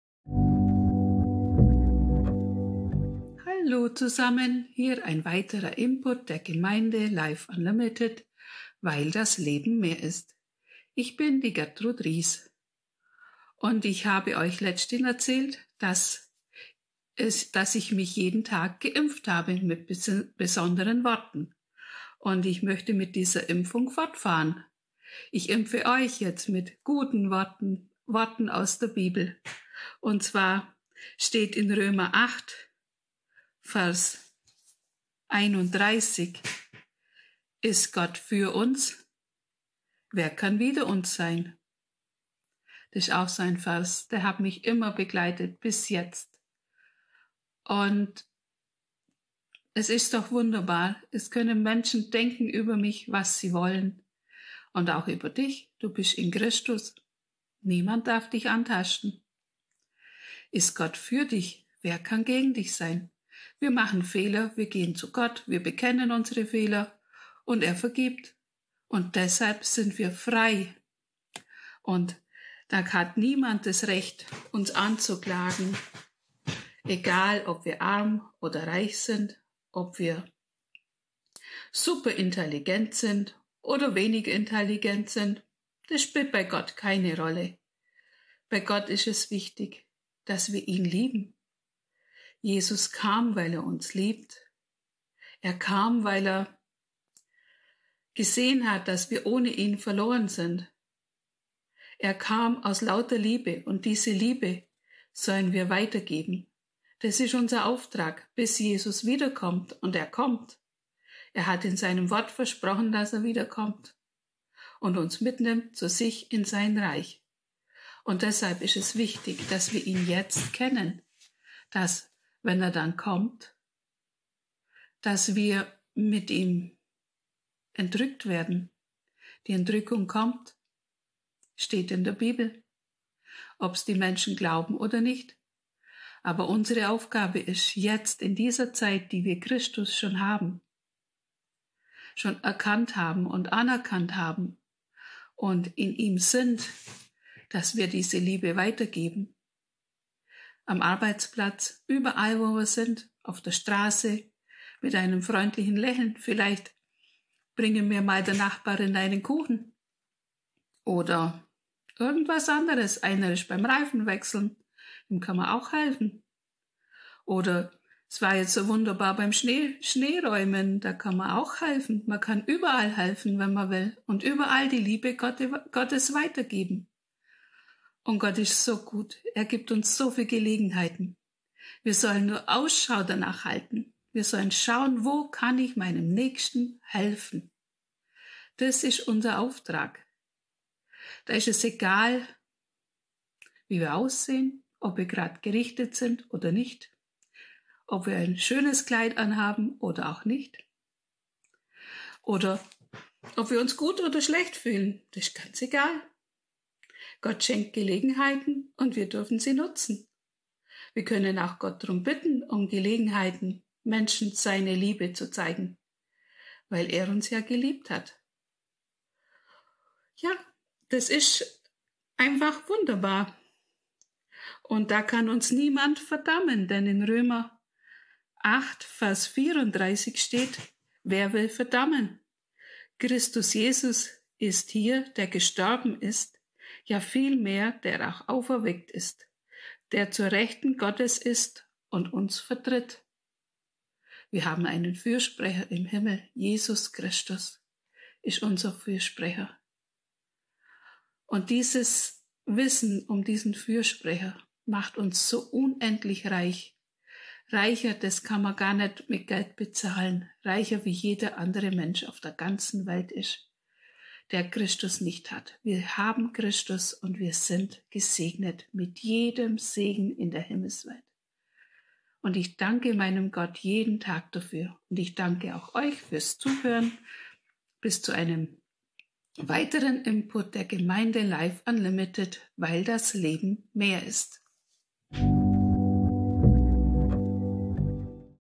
Alles dient dir | Input | 12. Februar 2021 | Life unlimited
Der Life unlimited Input am 12. Februar 2021 von Gertrud. Das Thema heute: Alles dient dir